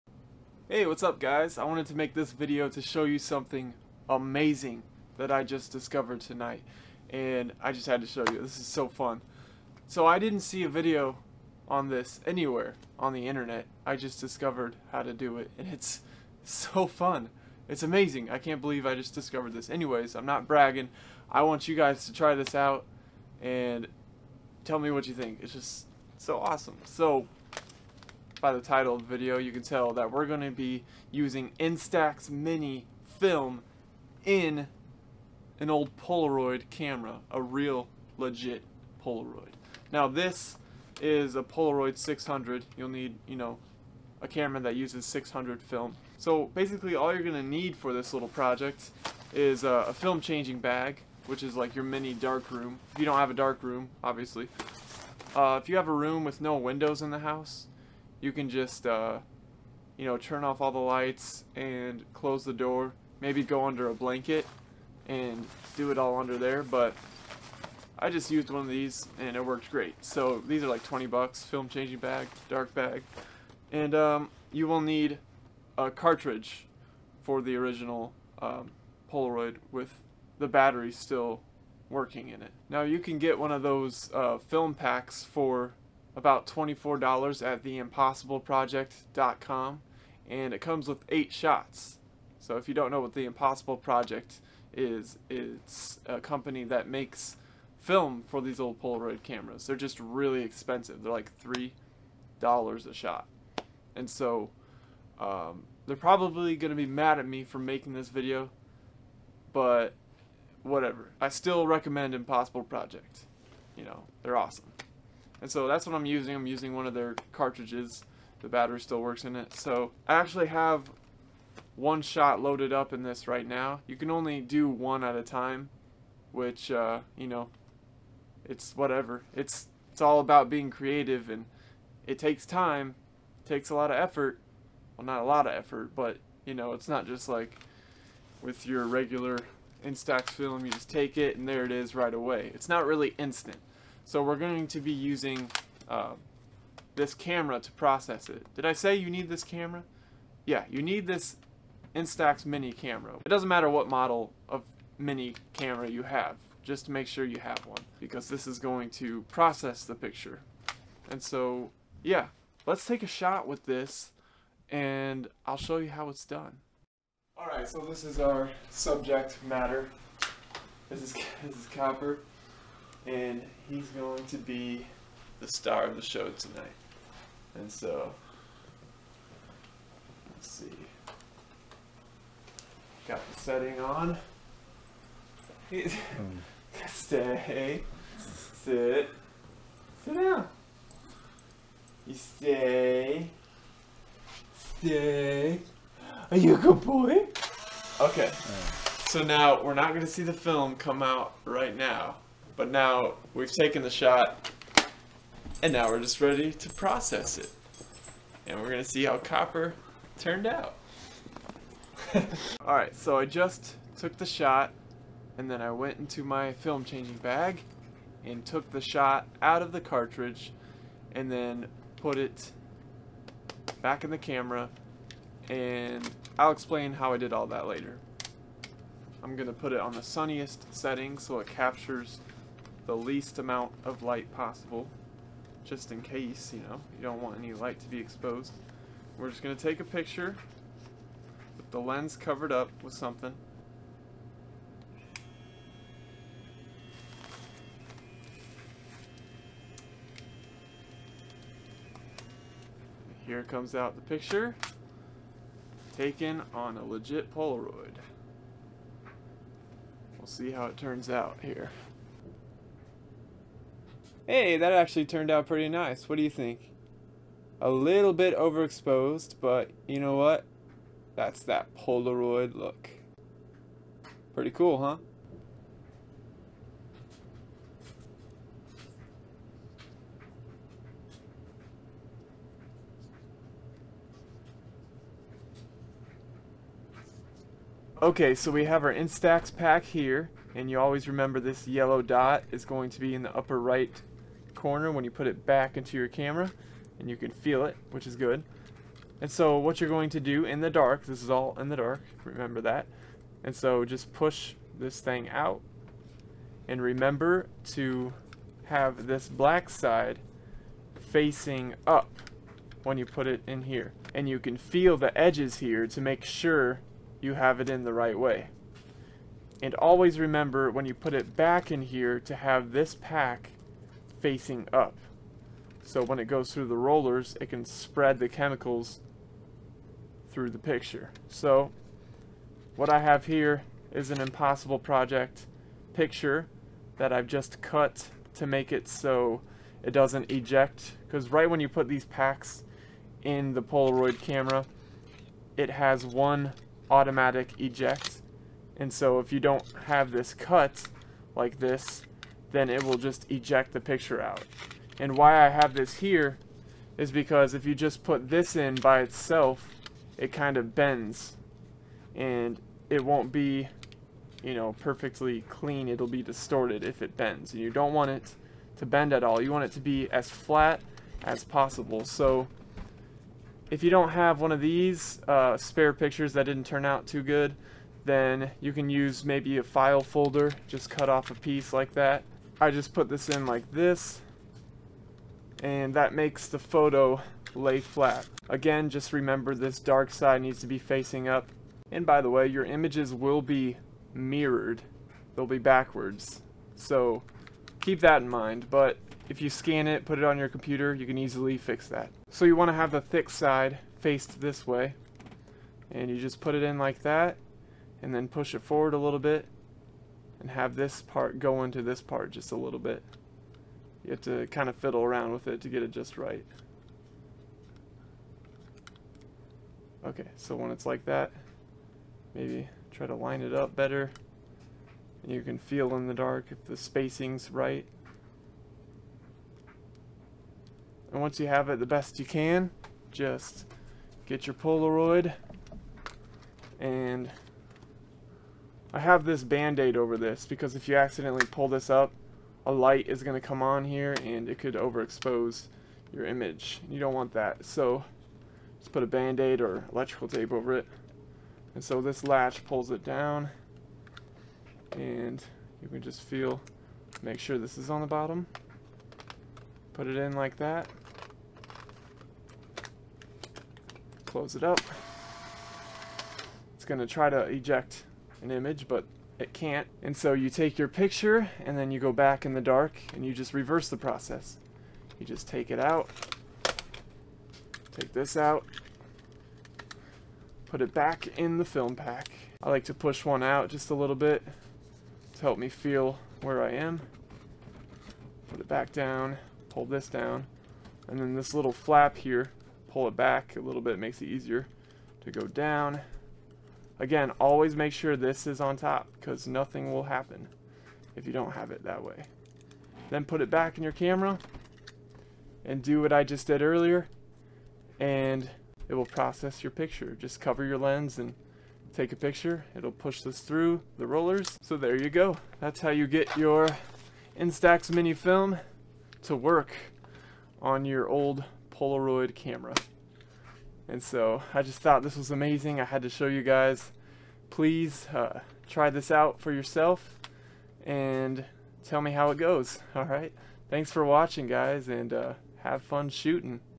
How To Use Instax Film In Polaroid
I found a way to use instax mini film in a Polaroid 600 camera! It's so much fun! I bet the instax wide would work with this method too.